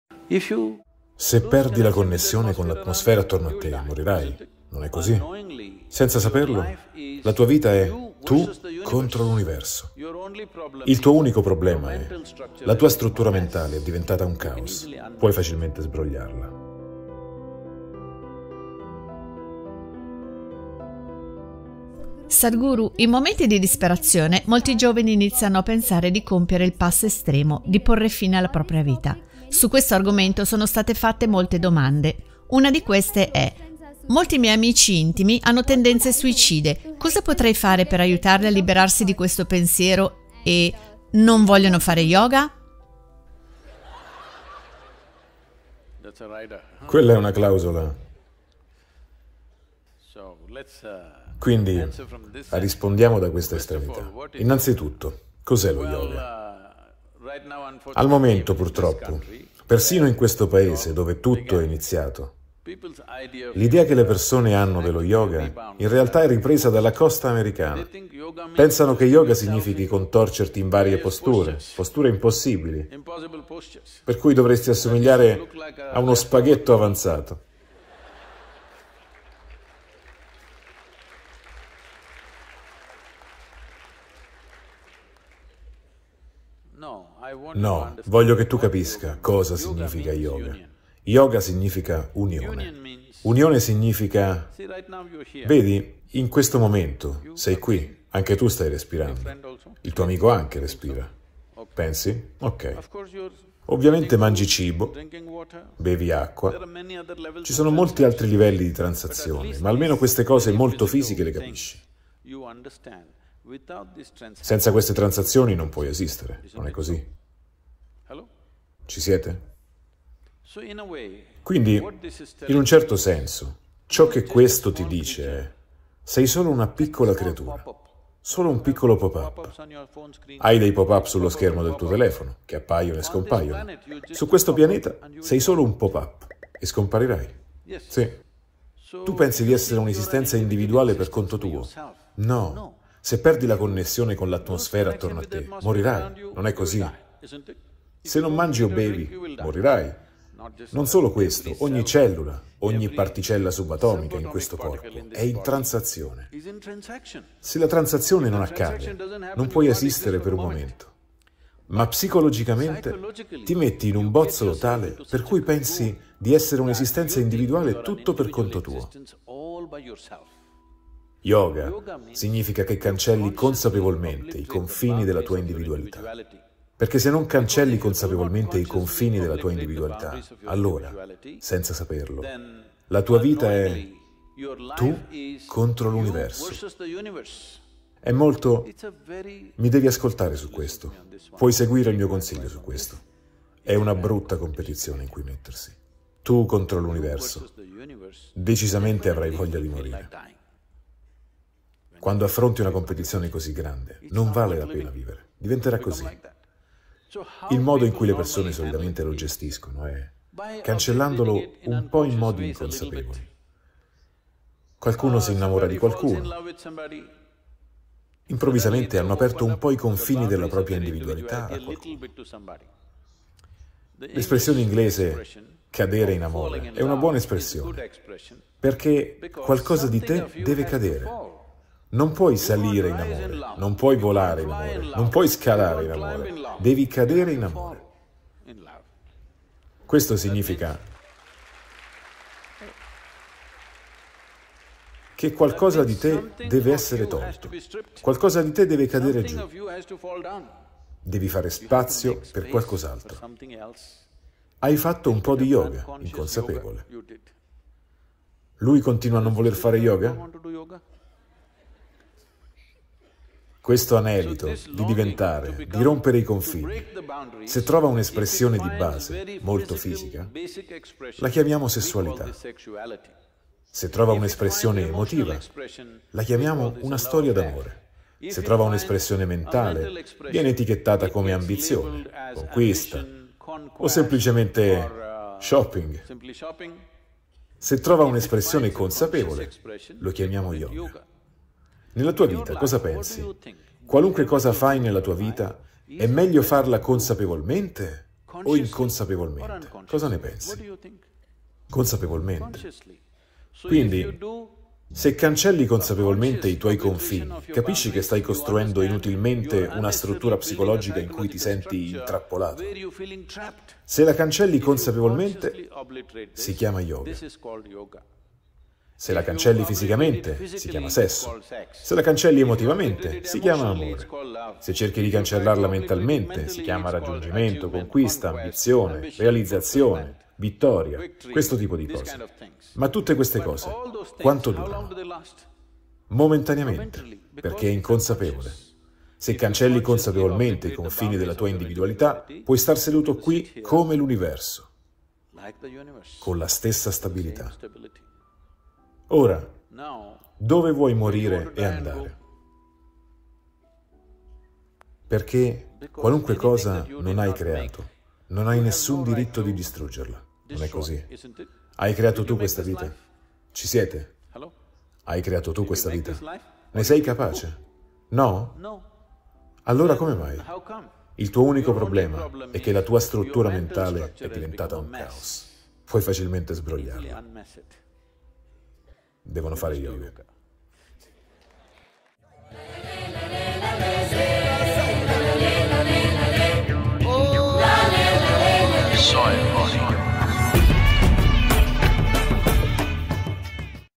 Come superare i pensieri suicidi? Sadhguru risponde | Sadhguru Italiano
Rispondendo alla domanda di una studentessa, Sadhguru spiega le basi dei pensieri suicidi. Sadhguru dà anche una prospettiva su come stabilire il proprio modo di essere in modo da poter eliminare completamente la confusione mentale.